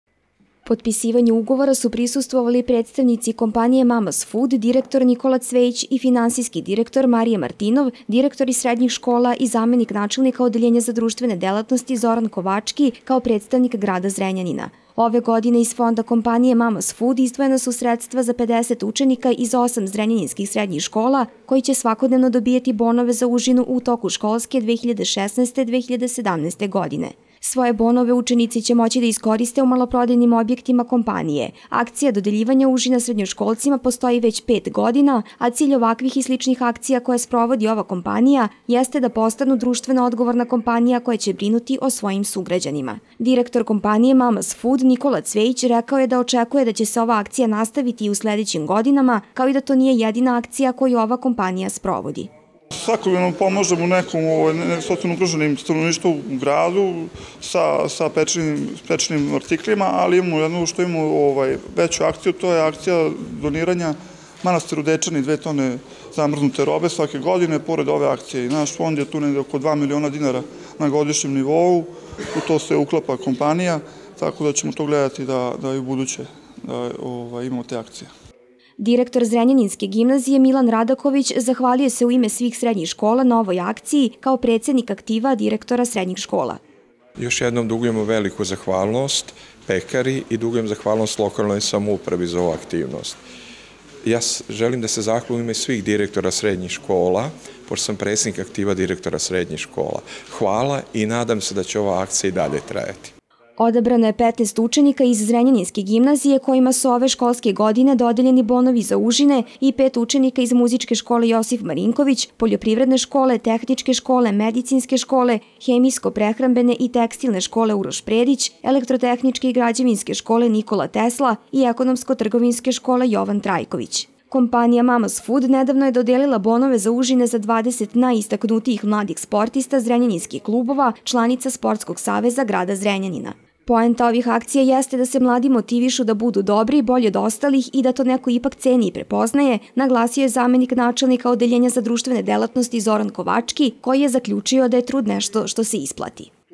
Potpisivanje ugovora o dodeli 50 užina preduzeća Mamma's Food
U Multimedijalnoj Sali Gradske kuće, preduzeće Mamma's Food potpisalo je ugovore o dodeli užina sa direktorima zrenjaninskih srednjih škola.